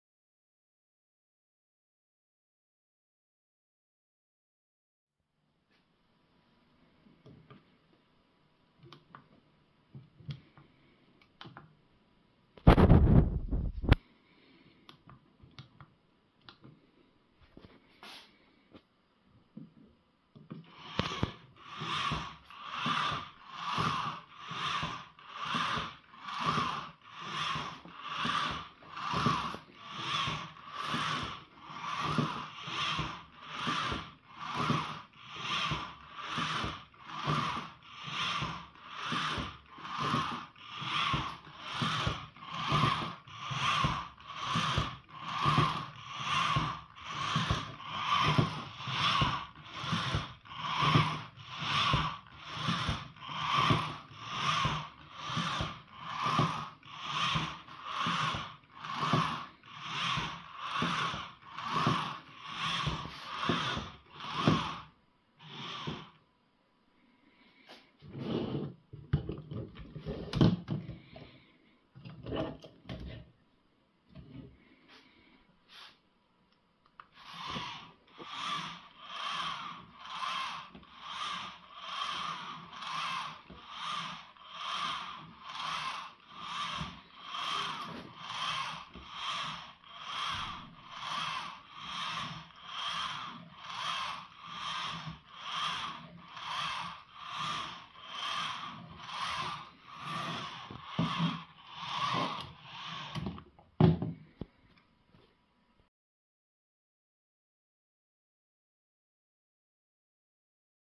Modular Pythonic Python v0.1
A modular snake/worm-like robot that uses Pybricks broadcasting feature.